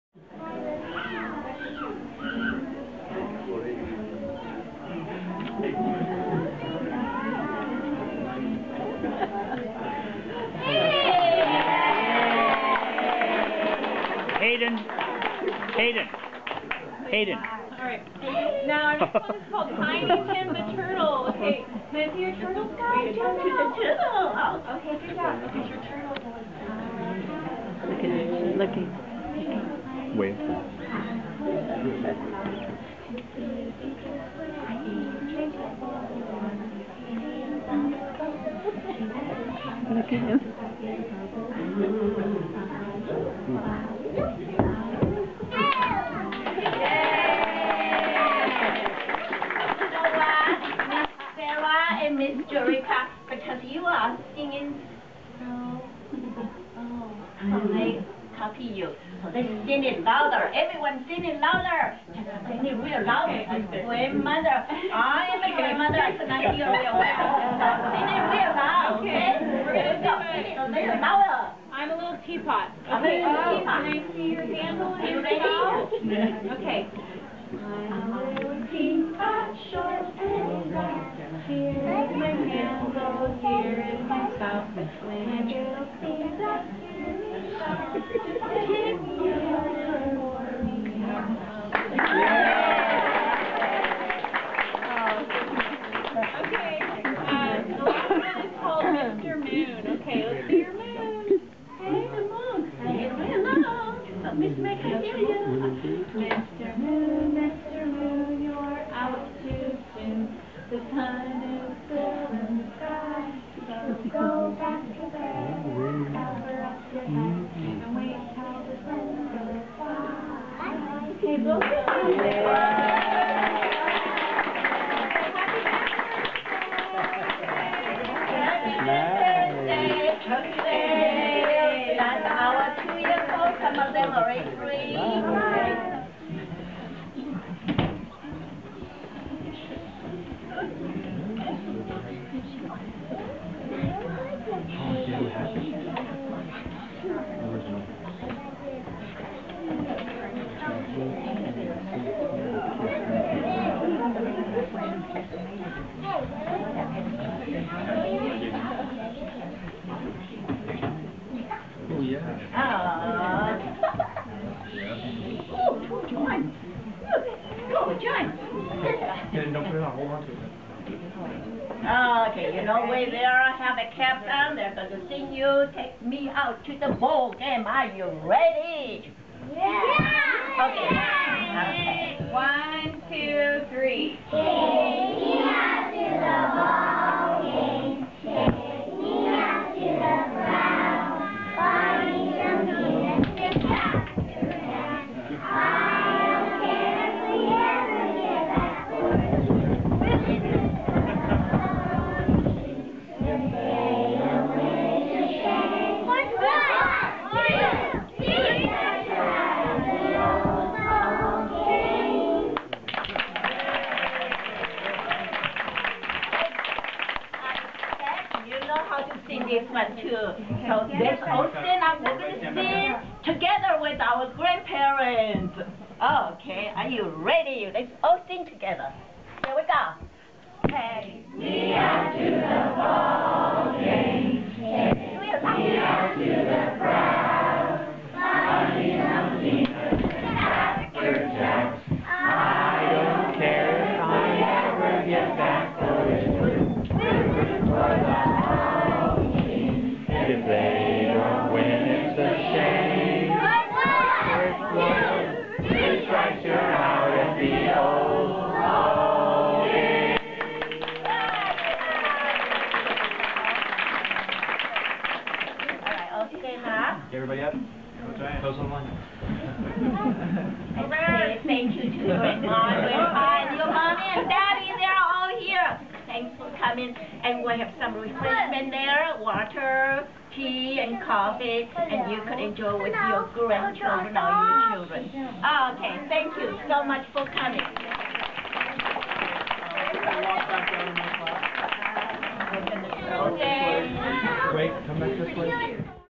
Grandparents Day 5-27-2011
Hayden & Michael performing at Happy Days on Grandparents Day 2011